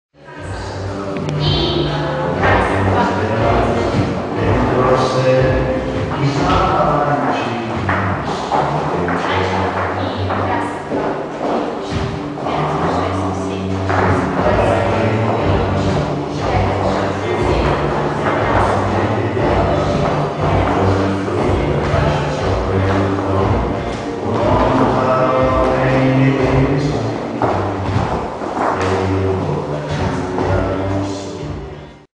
ТСК Big Step. Ритм-блюз
Группа Массовый спорт танцует танец Ритм-блюз.